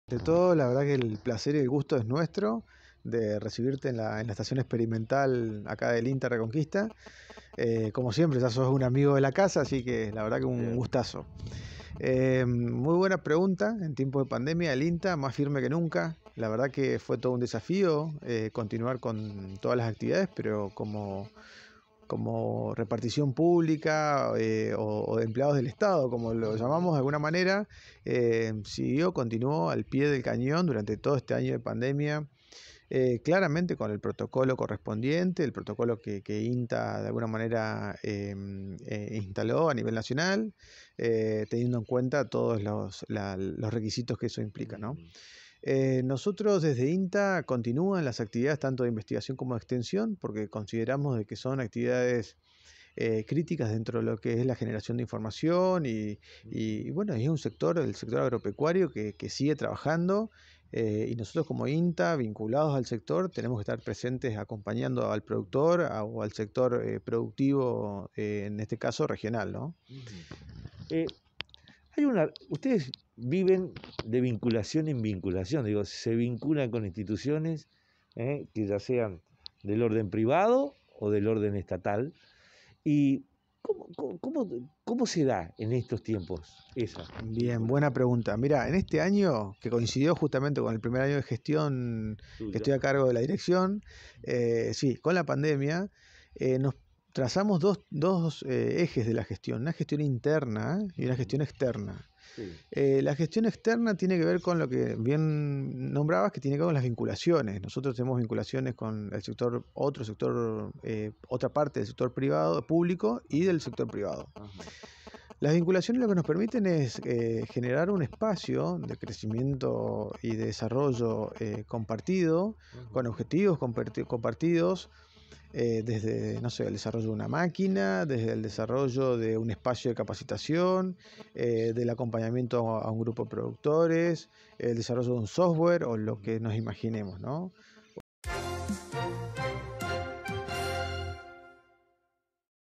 INSTITUCIONES - INTA EN TIEMPOS DE PANDEMIA - MARCELO PAYTAS – DIR. EEA RQTA – PARTE 1
DIALOGAMOS CON EL DIRECTOR DE LA ESTACION EXPERIMENTAL AGROPECUARIA INTA RECONQUISTA
EL ING. MARCELO PAYTAS Y RECORRIMOS LAS INSTALACIONES VIENDO COMO SE CONTINUA TRABAJANDO EN TIEMPOS DE PANDEMIA.